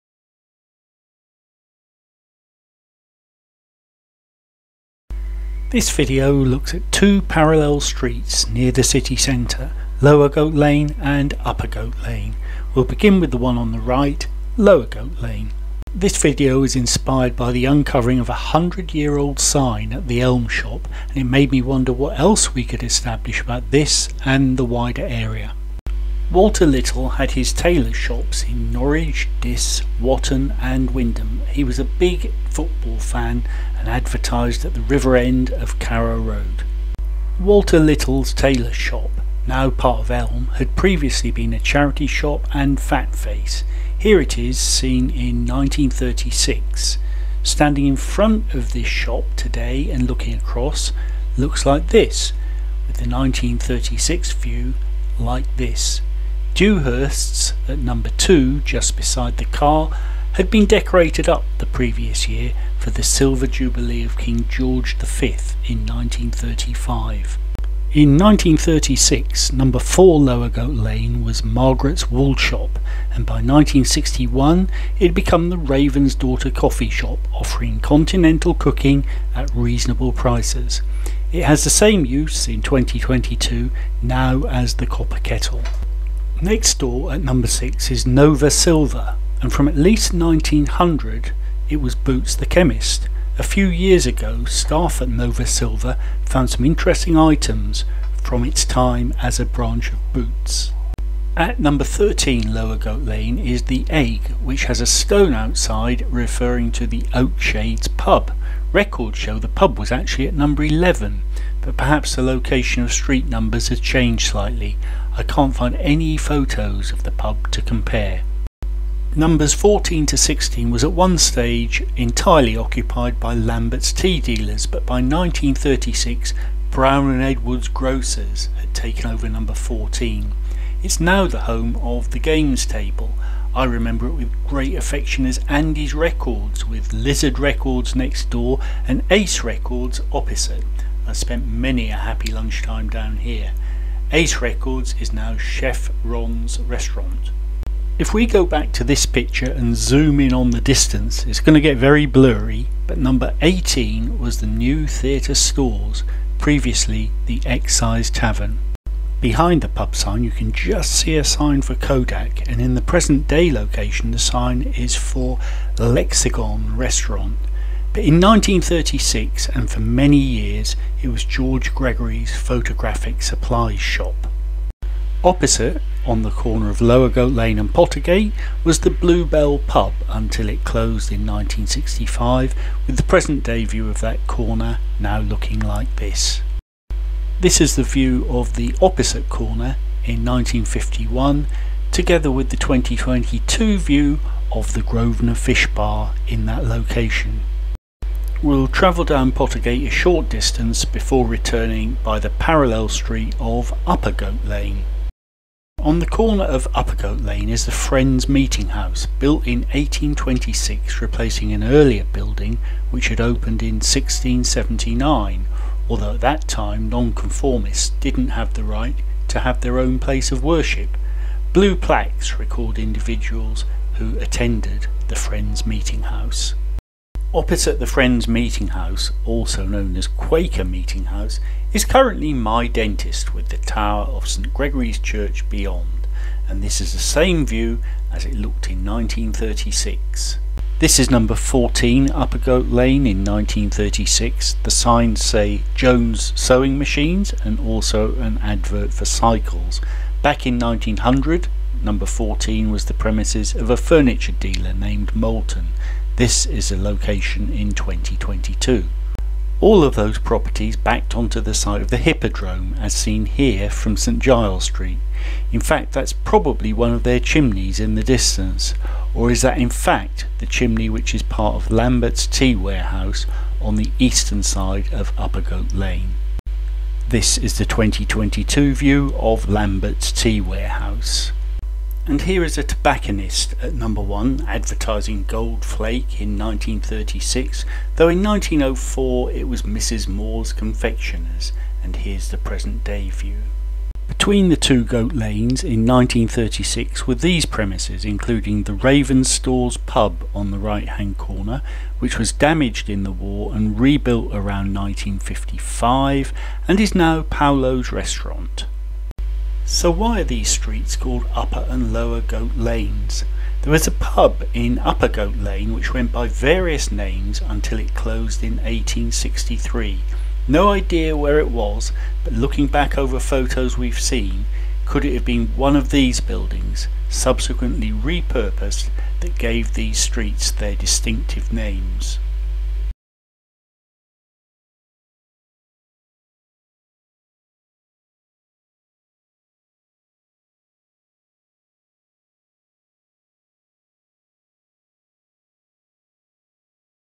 Lost Norwich - Lower Goat Lane and Upper Goat Lane
The uncovering of a 100 year old shop sign inspired this video about two parallel streets within the Norwich Lanes.